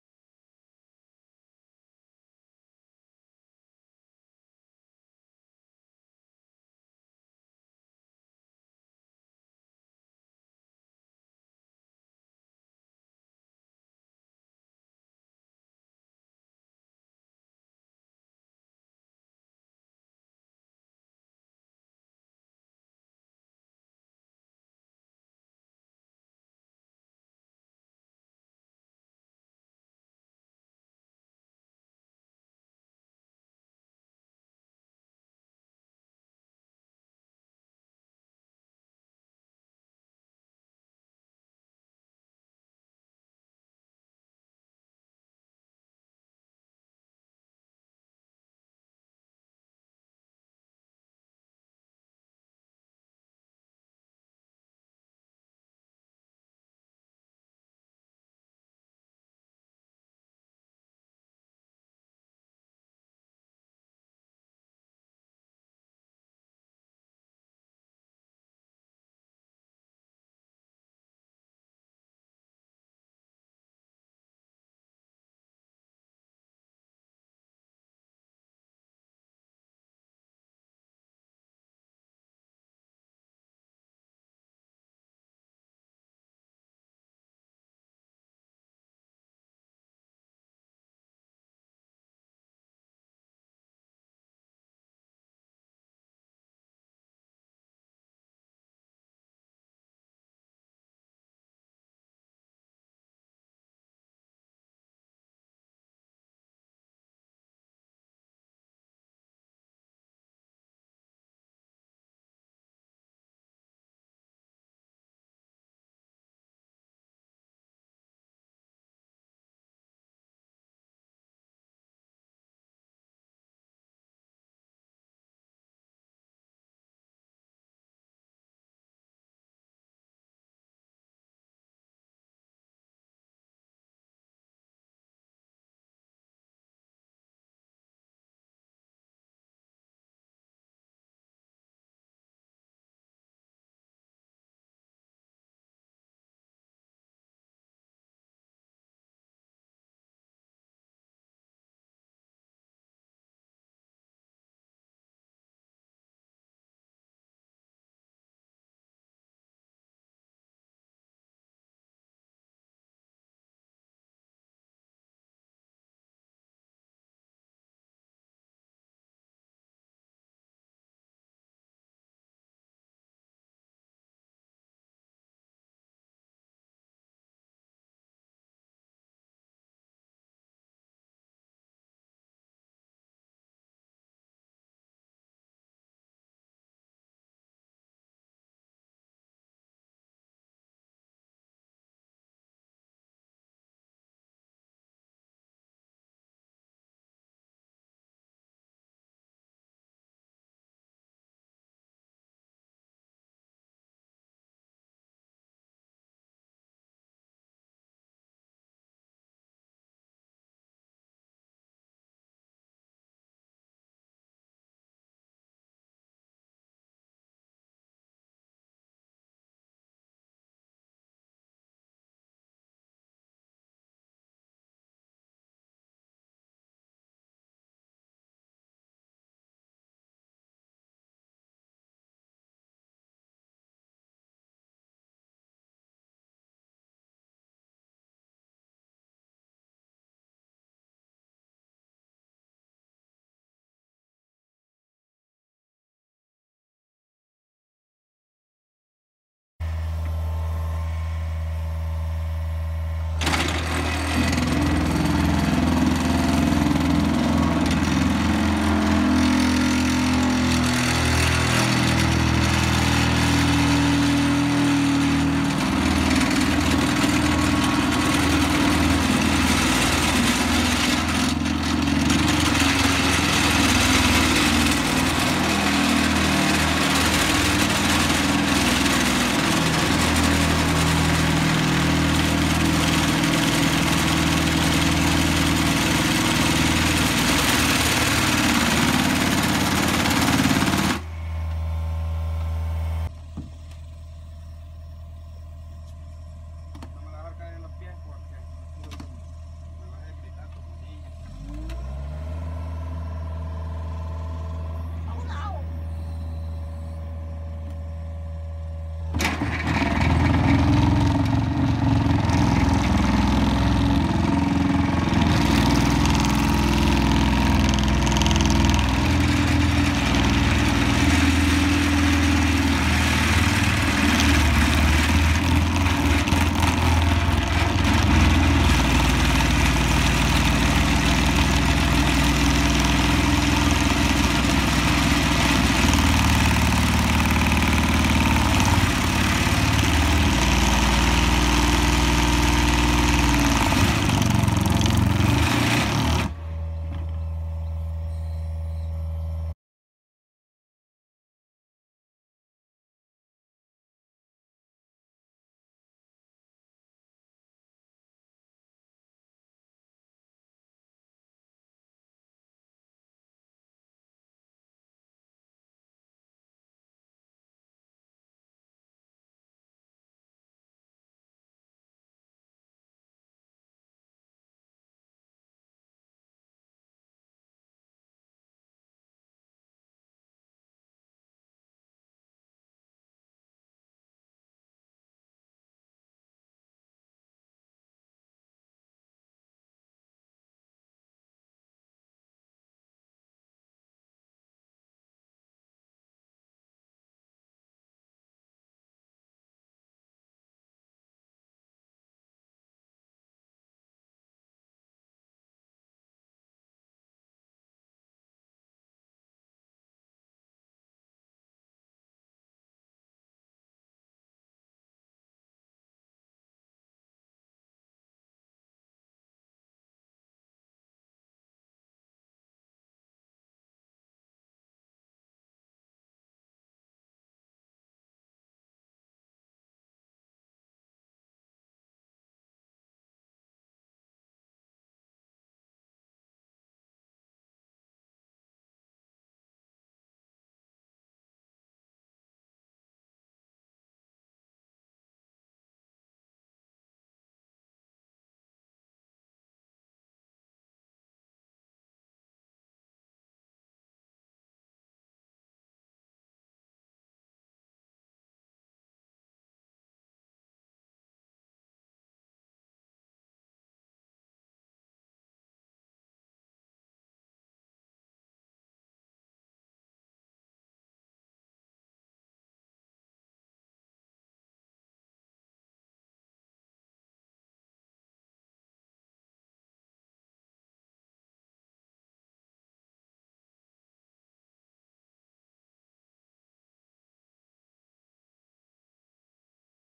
Lake Seawall Build
Replacing an old destroyed Seawall at Lake Tawakoni with a modern vinyl material.